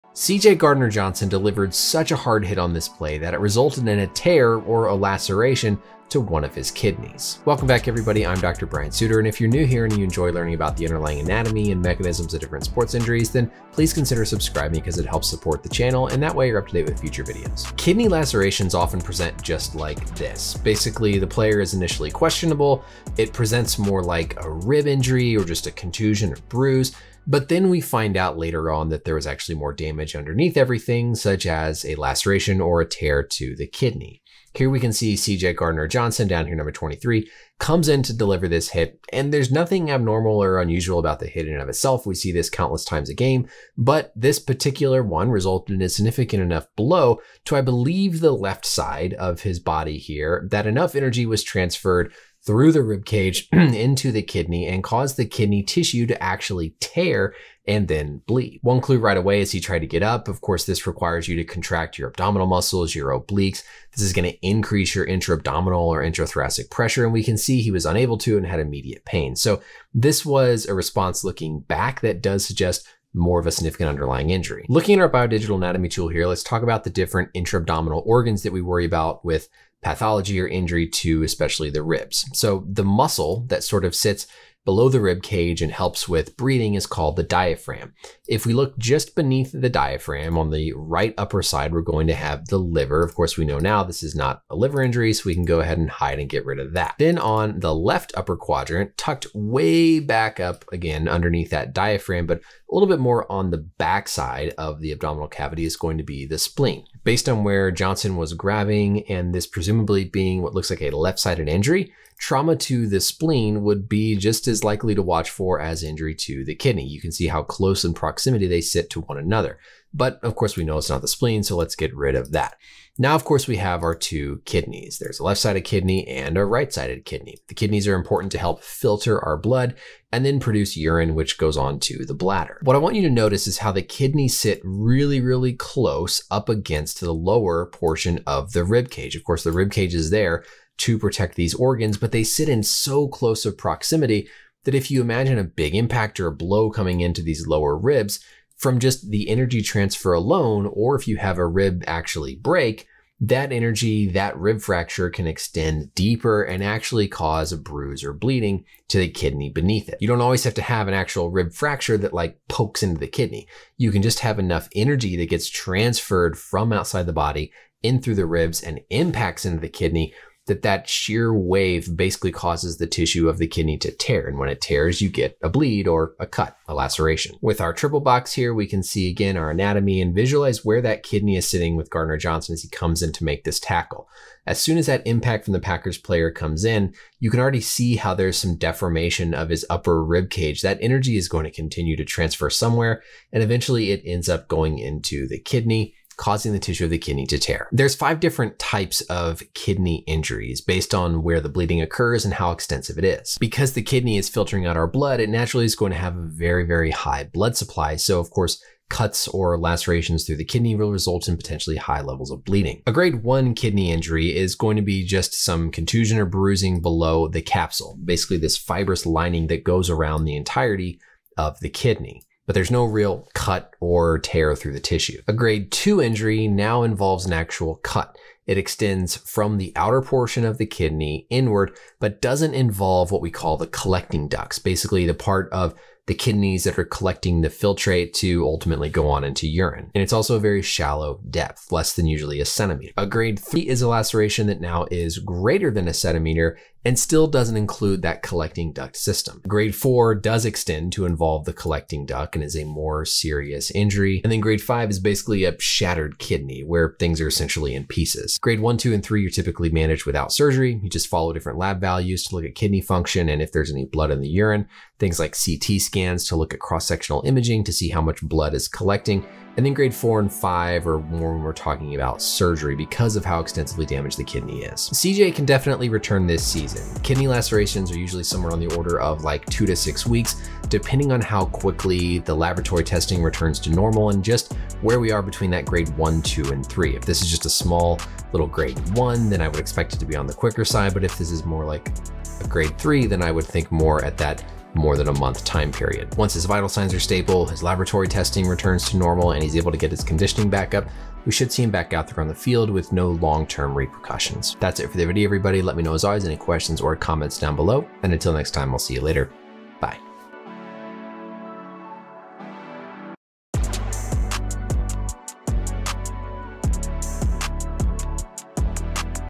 Hit SO HARD His Kidney Was TORN - Doctor Explains CJ Gardner Johnson NFL Injury
The Philadelphia Eagles star defensive back CJ Gardner Johnson suffered a kidney laceration in their Sunday Night game against the Green Bay Packers. What is this injury and how did it happen?